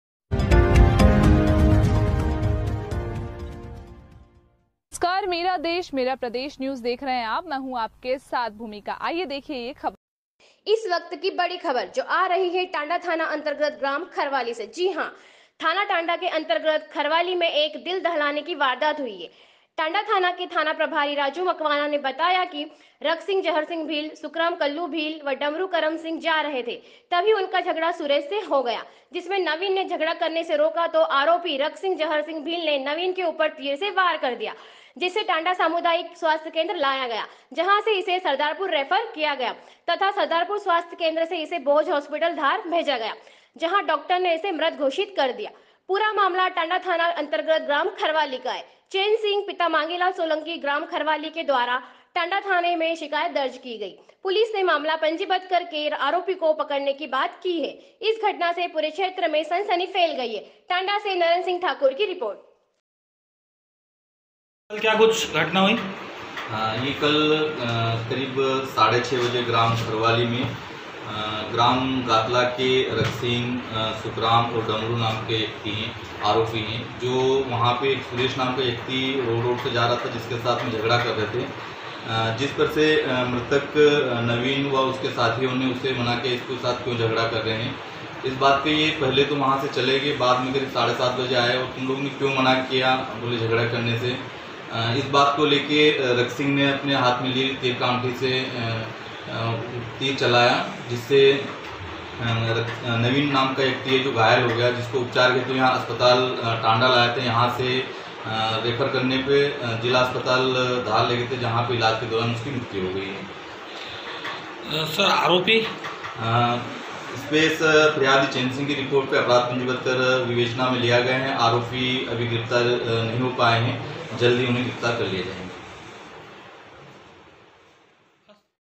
इस वक्त की बड़ी खबर थाना टांडा से झगड़ा रोकने की बात पर चलाया तीर है युवक की मौत
टांडा धार

इस वक्त की बड़ी खबर थाना टांडा से

चैन सिंह पिता मांगीलाल सोलंकी द्वारा टांडा थाना में शिकायत दर्ज की गई

झगड़ा रोकने की बात पर चलाया तीर है युवक की मौत

मामला टांडा थाना अंतर्गत ग्राम खरवाली  का है

टांडा से नरेंद्र सिंह ठाकुर की रिपोर्ट

Edit No. 1. Mavi Boy